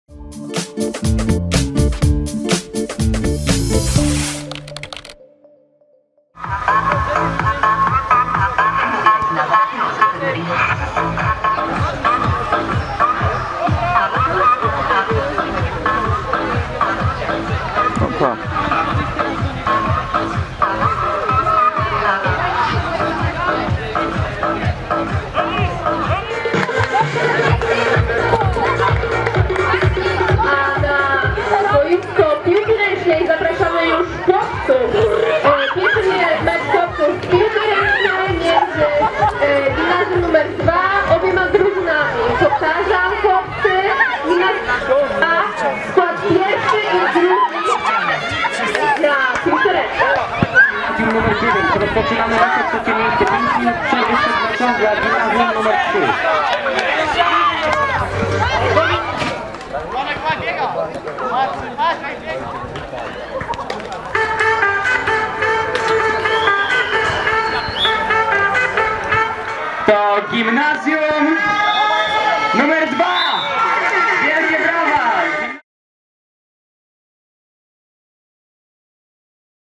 Uczniowie gimnazjów walczyli o medale na Kormoran Beach Party
Rozegrano XI Festiwal Plażowych Gier Zespołowych \"Kormoran Beach Party - Legnica 2016”. Gimnazjaliści rywalizowali w czterech dyscyplinach sportowych.